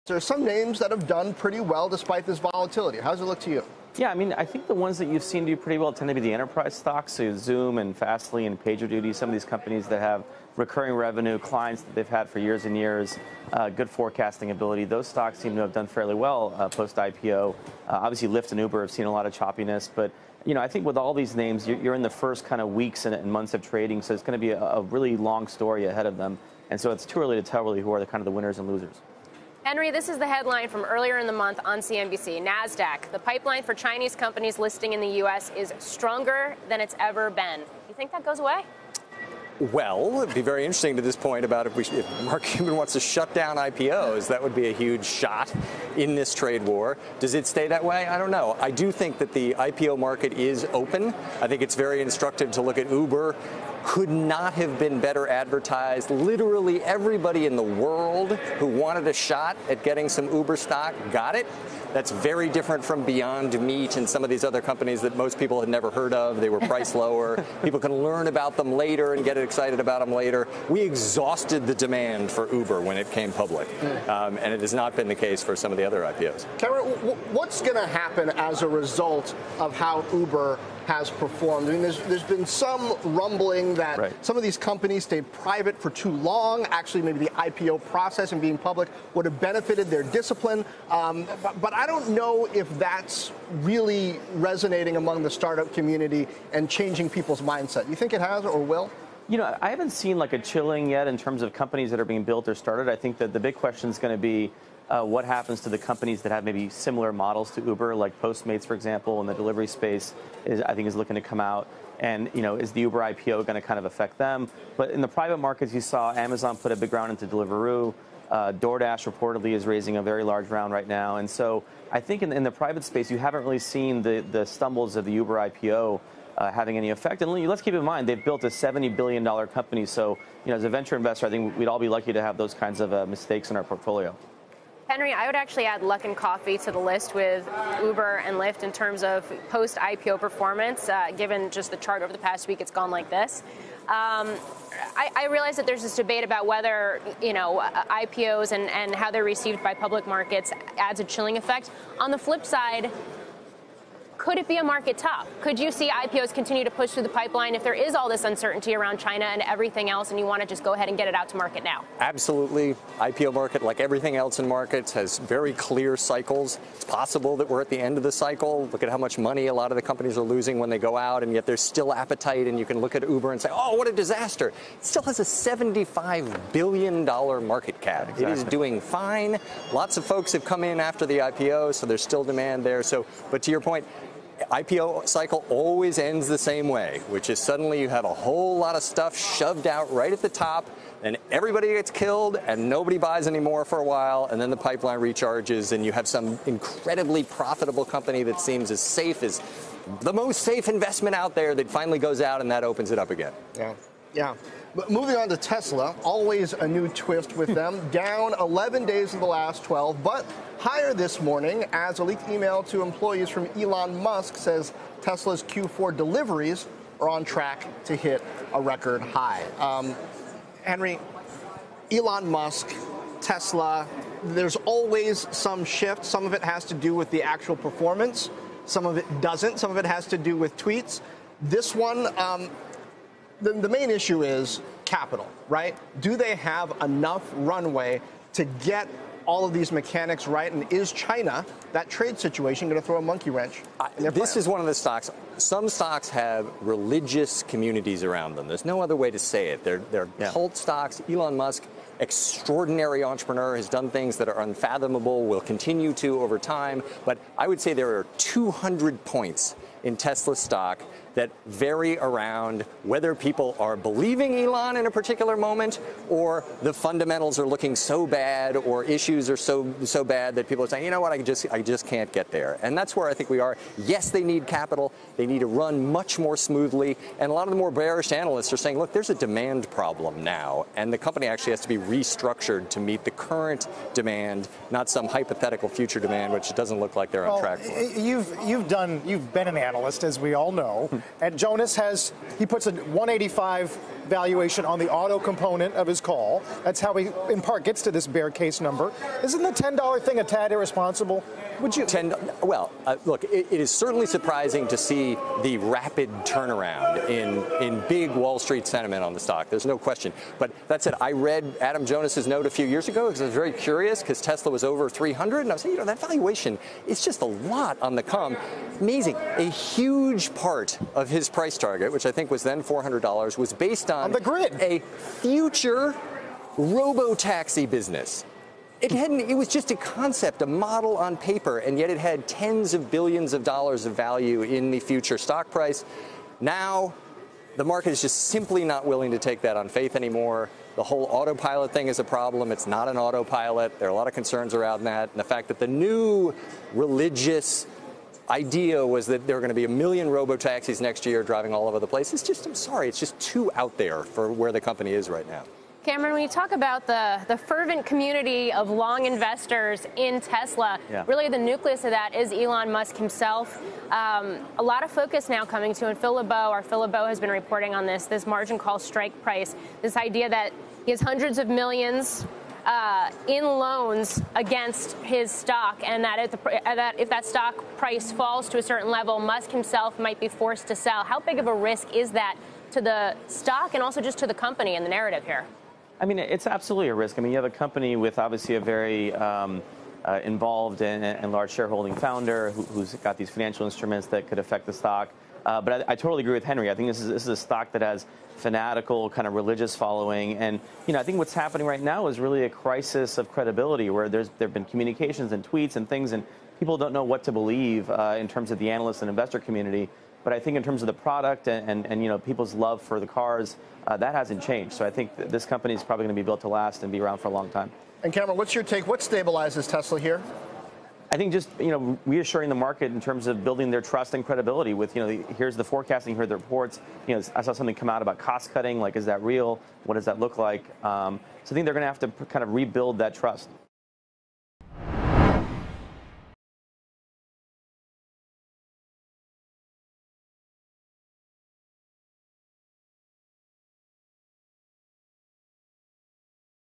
Enterprise IPOs seem to be doing well, says Greycroft's Kamran Ansari
Business Insider's Henry Blodget and Greycroft's Kamran Ansari discuss the winners and losers in the IPO rush.